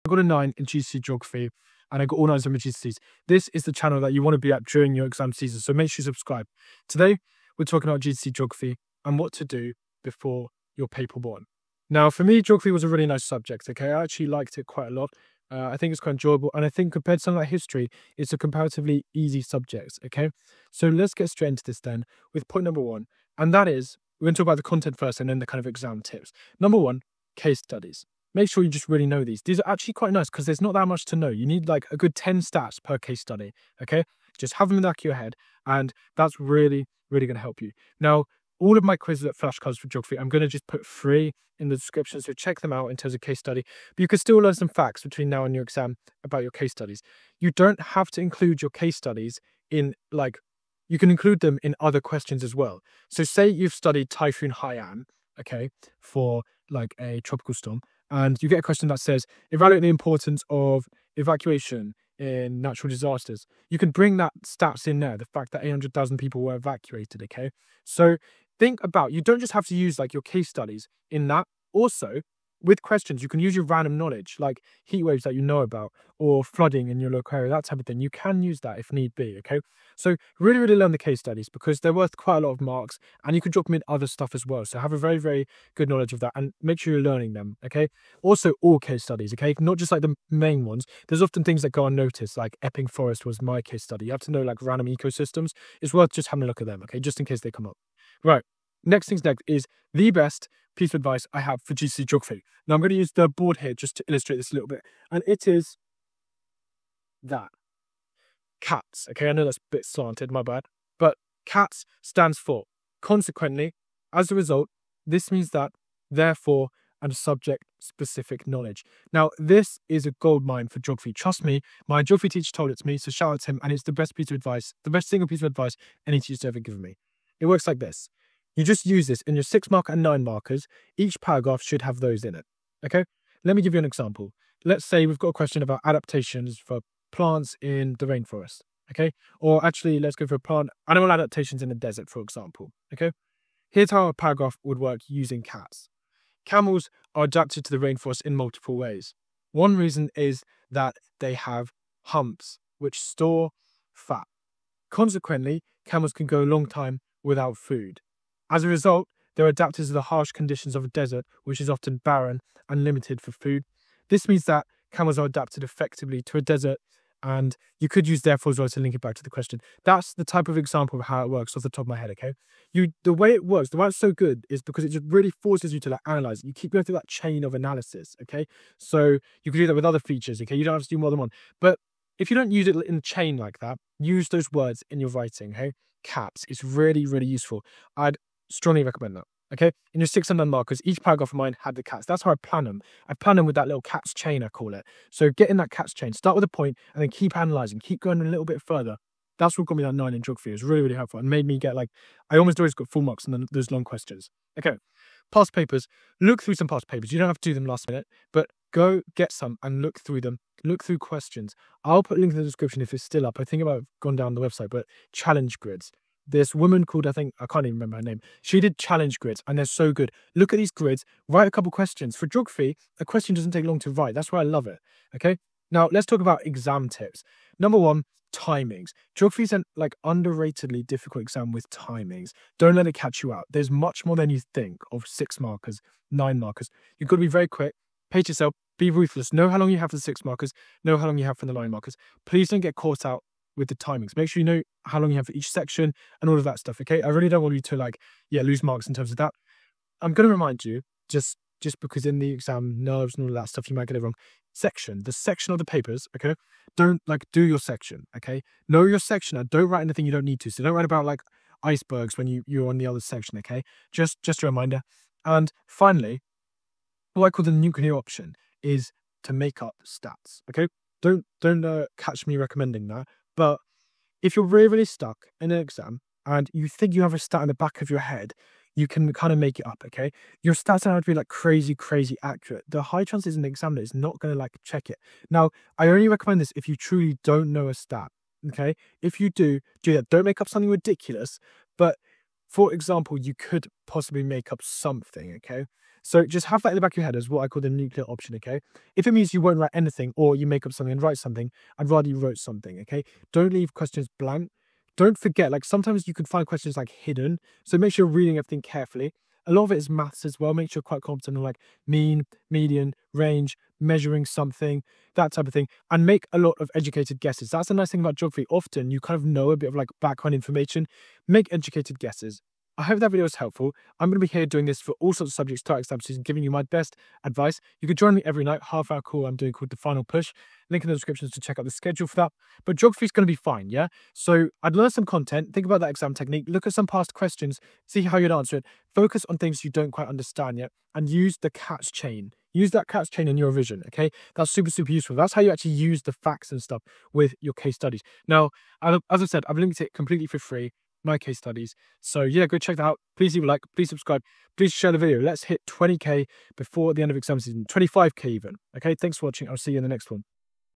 Watch This Video Before GCSE Geography Paper 1 | Free Resources
My FREE geography flashcards (not all topics are in here - I would use these for inspiration for your own flashcards).

A high mark CATTs paragraph:

In order to create the energy needed for billions of people, humans burn fossil fuels, as shown in figure 4. (e.g. coal, transport etc.). CO2 is released into the atmosphere. As a result, the greenhouse effect is enhanced as greenhouse gases reflect too much heat energy back to the Earth. This means that climate change becomes faster and therefore human fossil fuels for energy is a large human cause of climate change.

⌛TIMESTAMPS:
00:00 Introduction
00:28 Case studies
01:49 My best advice
03:25 Challenge grids
03:51 Timings
04:14 Sections
04:30 Make up facts if needed
05:09 Answer everything
05:17 Educated guesses
05:30 Important summary